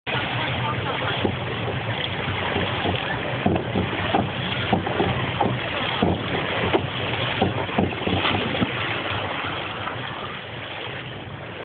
Göddy beim springen 3
Göddy macht ein Vorwärtssalto mit bombe
please kommis mache